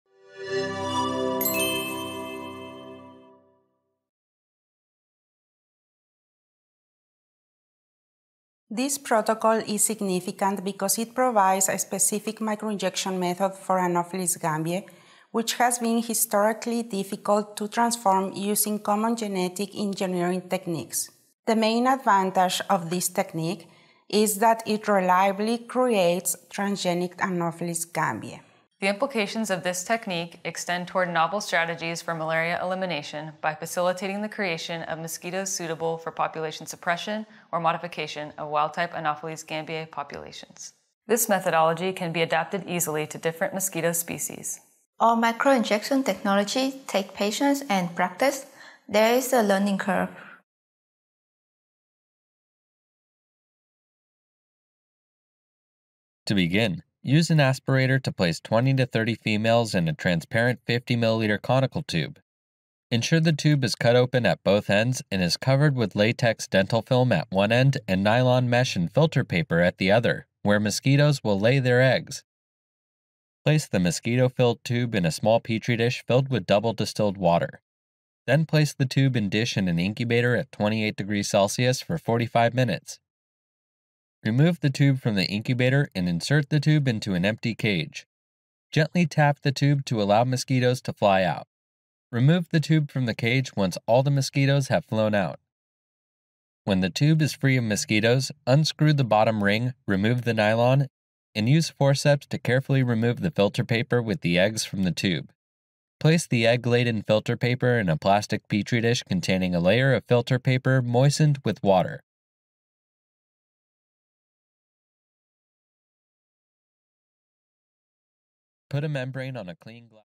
Microinjection of Anopheles gambiae Embryos | Protocol Preview
Microinjection Method for Anopheles gambiae Embryos -  a 2 minute Preview of the Experimental Protocol

Rebeca Carballar-Lejarazú, Taylor Tushar, Thai Binh Pham, Anthony A. James
University of California, Irvine, Department of Microbiology & Molecular Genetics; University of California, Irvine, Department of Molecular Biology & Biochemistry;

Microinjection techniques are essential to introduce exogenous genes into the genomes of mosquitoes. This protocol explains a method used by the James laboratory to microinject DNA constructs into Anopheles gambiae embryos to generate transformed mosquitoes.